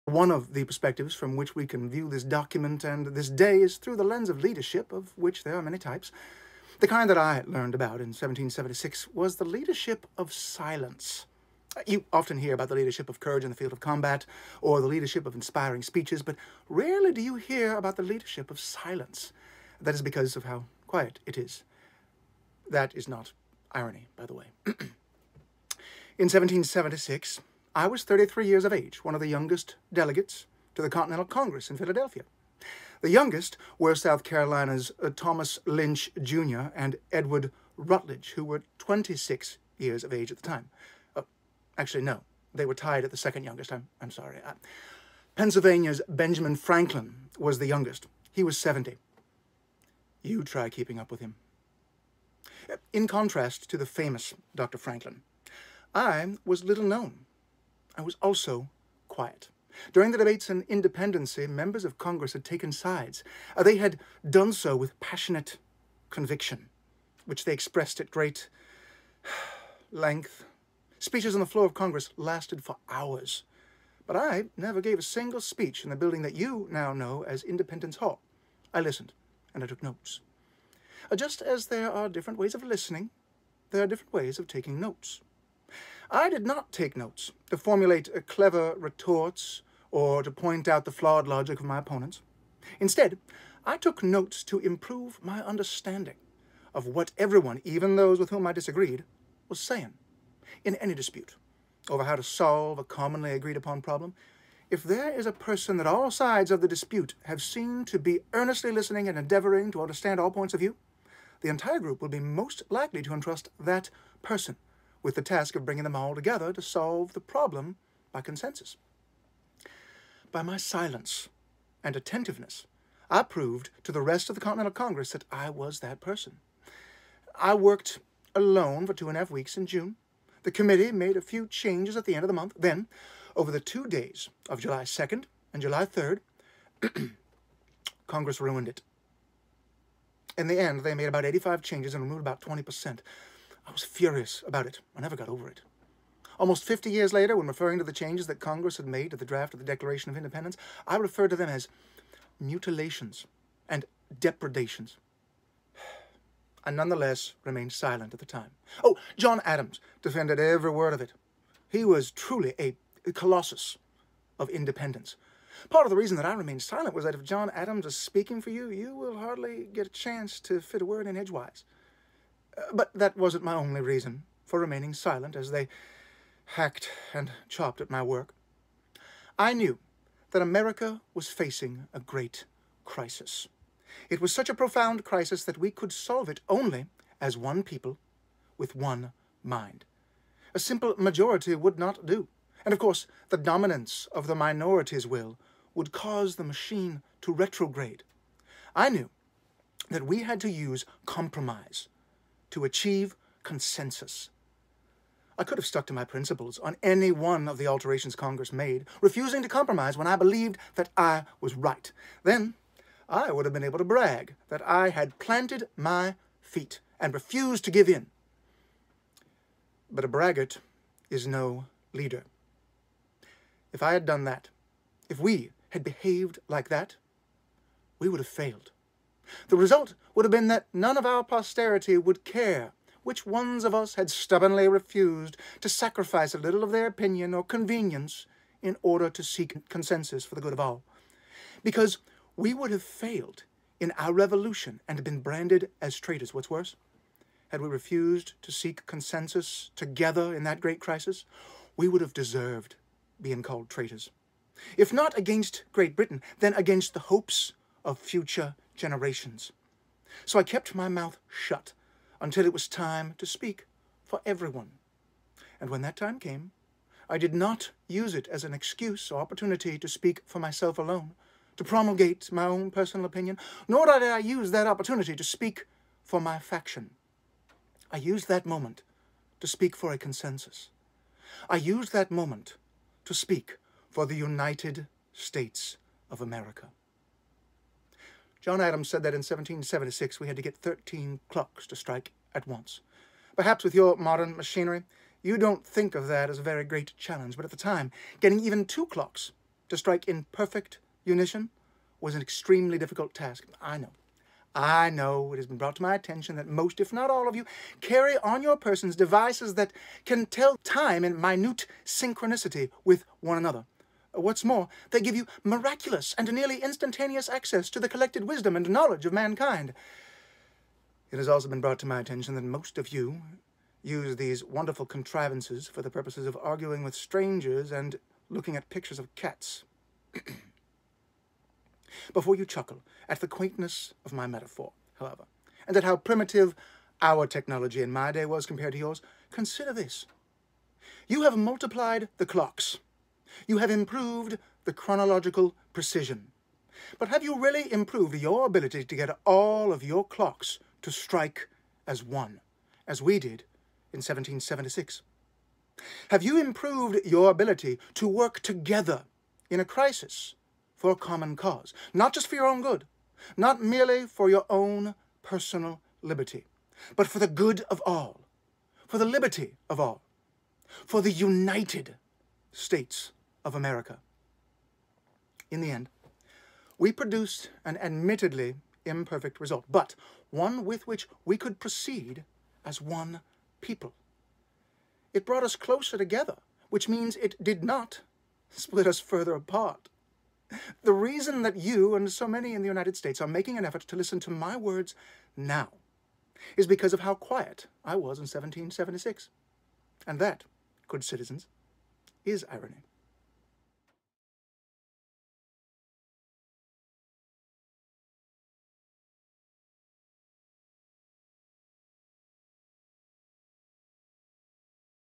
Thomas Jefferson shares leadership lessons from The Declaration of Independence
Thomas Jefferson (Steve Edenbo) discusses a type of leadership —"the leadership of silence"— that he learned about during the process of writing, editing, and voting for The Declaration of Independence in 1776.